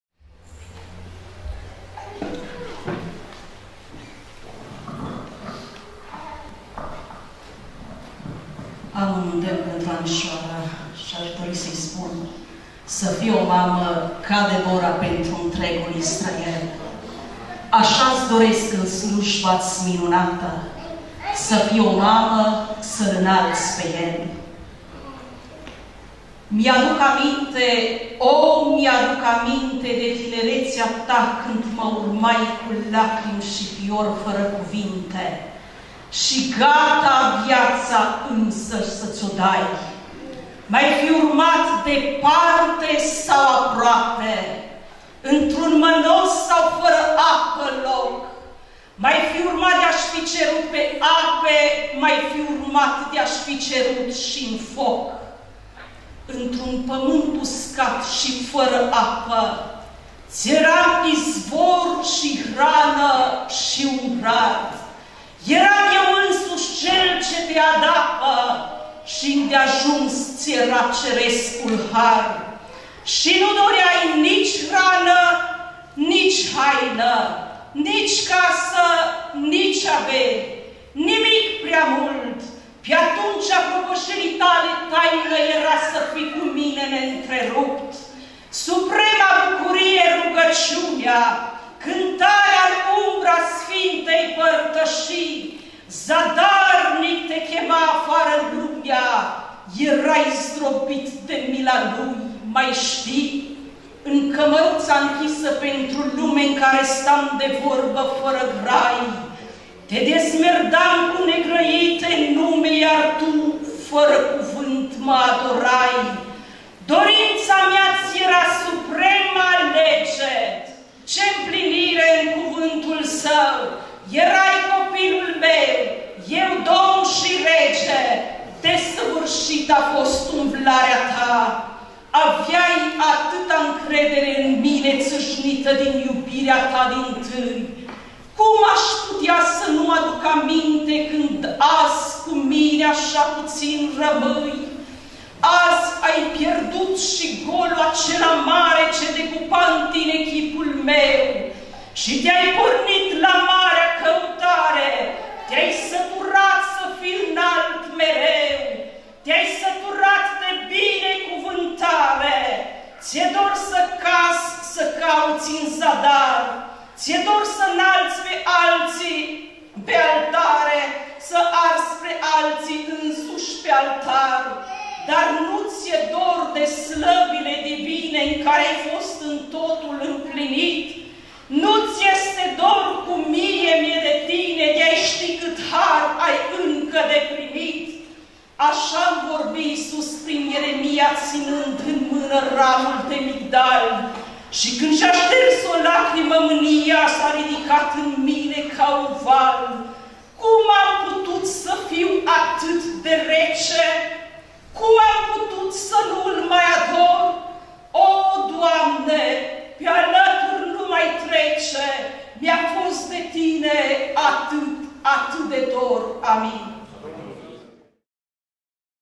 Lidia Creta - Așa-mi vorbi Isus
Lidia Creta
Biserica Carmel Birmingham

Poezia : Așa-mi vorbi Isus
"Mi-aduc aminte, o, mi-aduc aminte,
De tinerețea ta, când Mă urmai
cu lacrimi și fior, fără cuvinte,
și gata, viața însăși să Mi-o dai...

M-ai fi urmat, departe sau aproape,
într-un mănos sau fără apă loc...
M-ai fi urmat, de-aș fi cerut pe ape...
M-ai fi urmat, de-aș fi cerut, și-n foc...

Într-un pământ uscat și fără apă,
ți-eram Izvor și Hrană și Umbrar...
Eram Eu Însumi Cel ce te adăpă
și îndeajuns ți-era cerescul har...

Și nu doreai nici hrană și nici haină,
nici casă, nici averi, nimic prea mult.
Pe-atunci, a propășirii tale haină
era să fii cu Mine ne-ntrerupt.

Suprema bucurie, rugăciunea,
cântarea-n umbra sfintei părtășii.
Zadarnic te chema afară Lumea.
Erai zdrobit de mila ei.
Mai știi...?

În cămăruța-nchisă pentru Lume,
în care stam de vorbă fără grai,
te dezmierdam cu negrăite nume,
iar tu...fără cuvânt Mă adorai...

Dorința Mea ți-era suprema lege.
Ce fericire-n împlinirea Sa!
Erai copilul Meu.
Eu - Domn și Rege.
Desăvârșită-a fost umblarea ta!

Aveai atâta-ncredere în Mine,
țâșnită din iubirea ta dintâi!
Cum aș putea să nu-Mi aduc aminte,
când azi, cu Mine-așa puțin rămâi!

Azi ai pierdut și golu-acela mare
ce decupa în tine chipul Meu
și te-ai pornit la marea căutare...
Te-ai săturat să fii înalt mereu,

Te-ai săturat de binecuvântare.
Ți-e dor să cazi, să cauți în zadar
Ți-e dor să-nalți din când în când altare,
să arzi spre alții, însuți, pe altar.

Dar nu ți-e dor de slăvile divine
în care-ai fost în totul împlinit...?
Nu-ți este dor, cum Mie Mi-e de tine?
De-ai ști cât Har ai încă de primit...! "
Așa-mi vorbi Isus prin Ieremia,
ținând în mână ramul de migdal
și când și-a șters o lacrimă, mânia
s-a ridicat în mine ca un val:

Cum am putut s-ajung atât de rece?
Cum am putut să nu Îl mai ador?
O, Doamne, pe alături nu mai trece!
Mi-a fost de Tine-atât...atât de dor!

9 May 2021